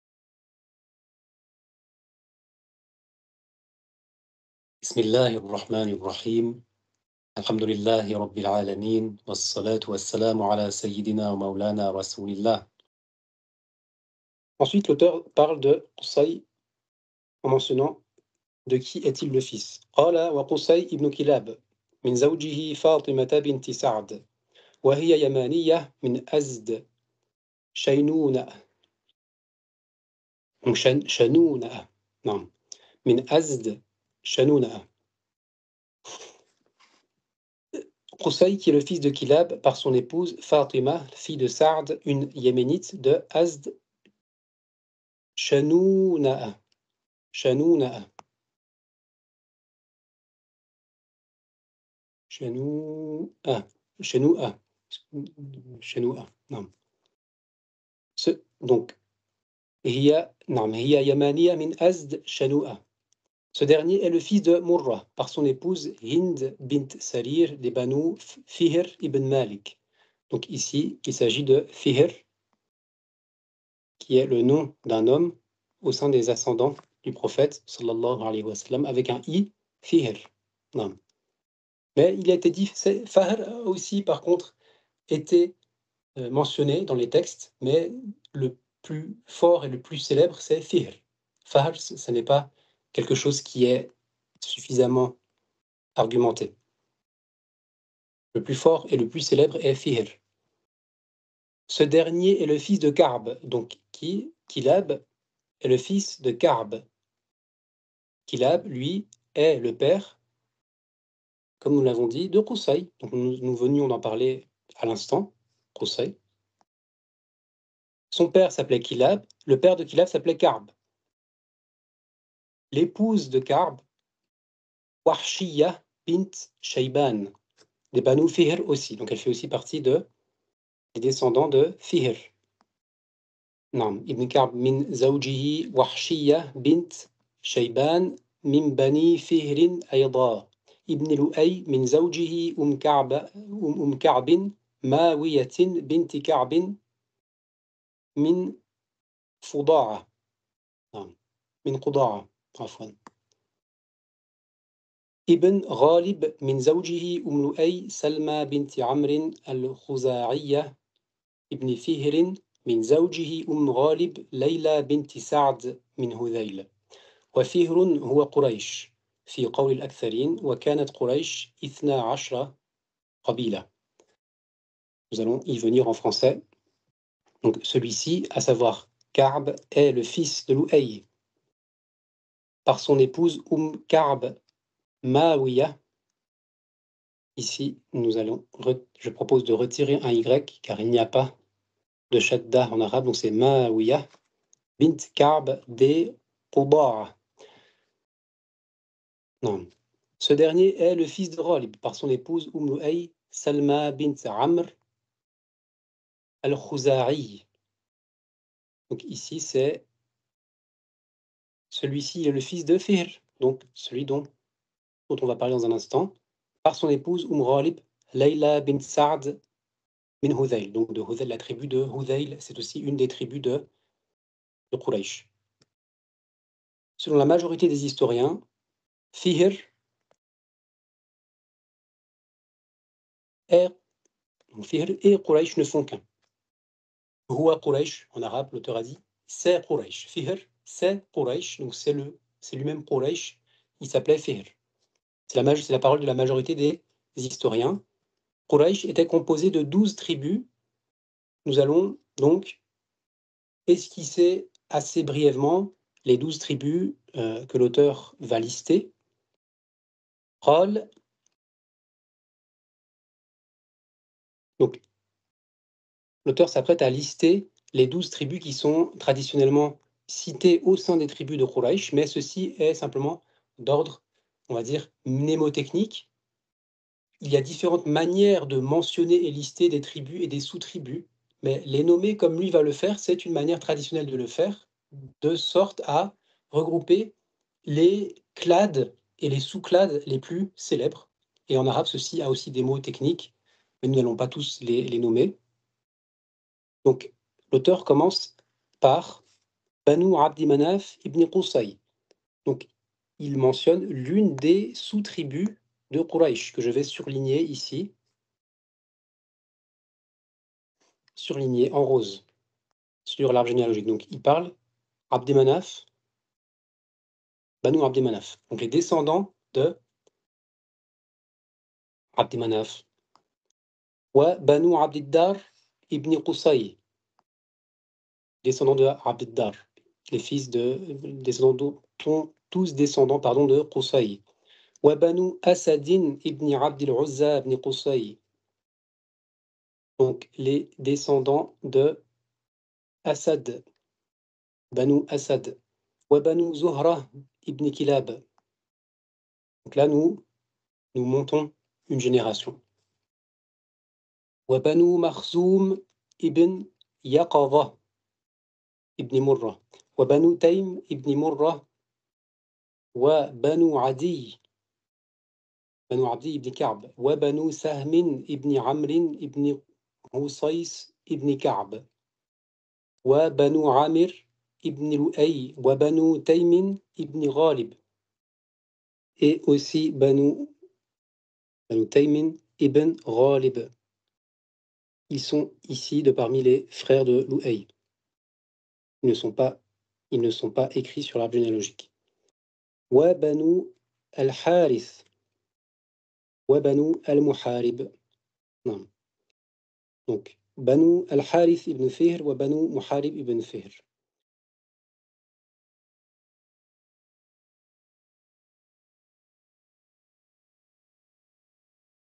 La généalogie bénie [18] - Rôle de Qusayy (3)
Nūr l-yaqīn ilā sīrati sayyidi l-mursalīn ﷺ

« Lumière de la certitude sur la biographie du Maître des envoyés » ﷺ
de son auteur, Muḥammad ibn Afīfī al-Khuḍarī